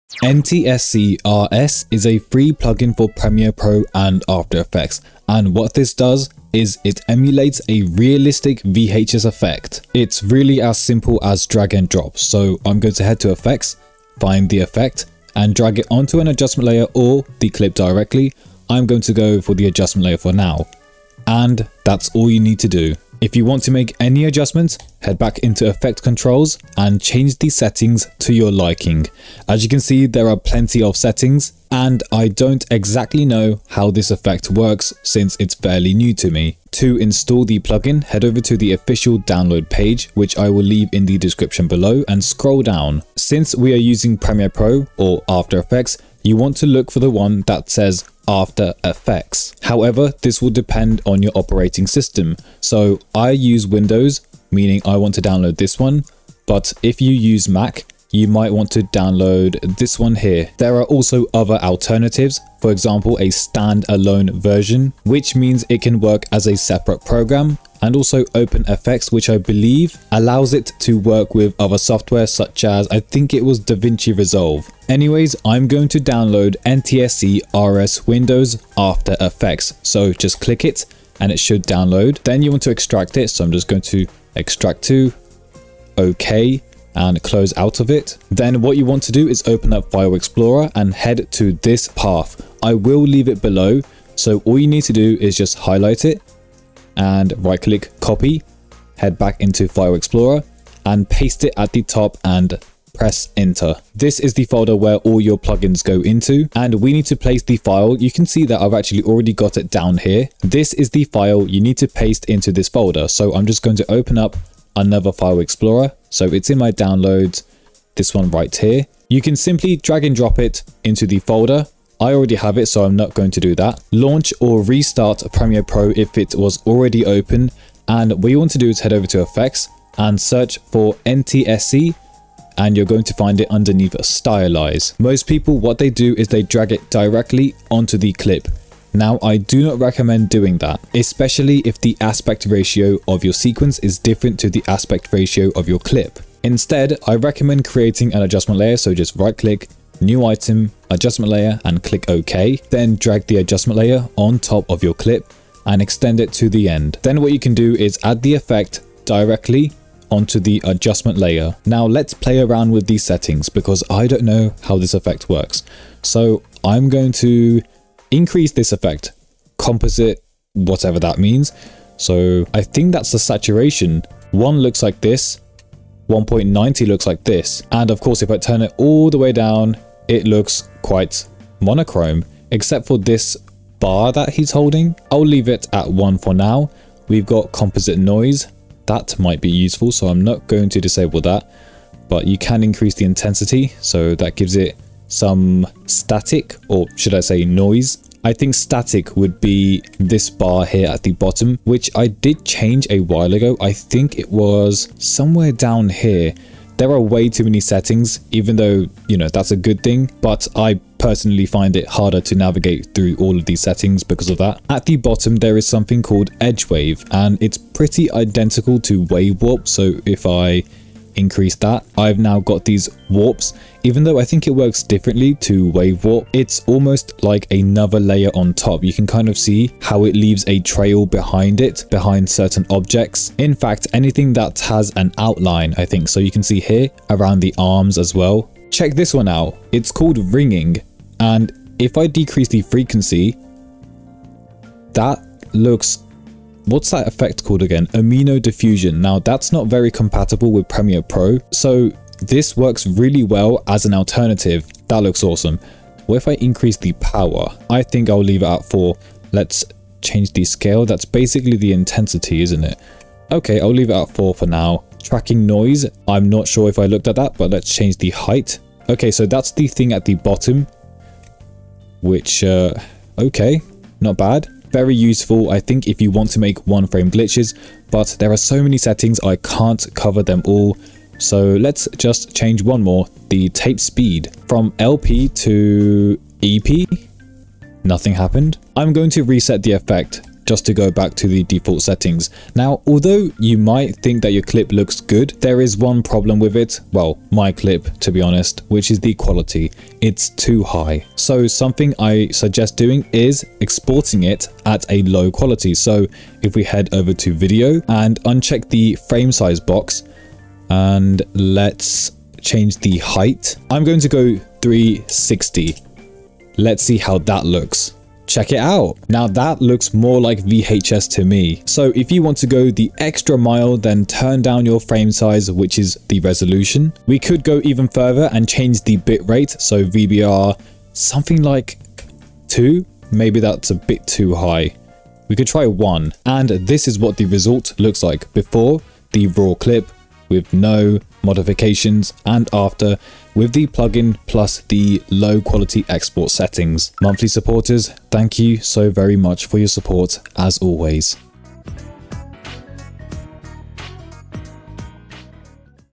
Realistic VHS Using ONE Effect! | Premiere Pro / After Effects
Introducing ntsc-rs! A completely free plugin for Premiere Pro, After Effects, Davinci Resolve and more! ntsc-rs emulates a VHS effect realistically - so if you're looking for a free plugin or an alternative to paid plugins (such as Universe/uni.vhs & the Signal plugin), then you NEED this! ntsc-rs can also be installed as a standalone program, so you don't need software such as Premiere Pro installed to use this plugin.

🖥️ Path for Windows: C:\Program Files\Adobe\Common\Plug-ins\7.0\MediaCore\

💻 Path for Mac: /Library/Application Support/Adobe/Common/Plug-ins/7.0/MediaCore/

🗣 SOCIALS!

⏰ Timestamps:

0:00 Overview
0:36 Downloading ntsc-rs
1:22 Extract & copy plugin
2:04 Using ntsc-rs
2:41 Settings (quick overview)
4:52 Enhancing the 'VHS effect'
5:36 RESULT!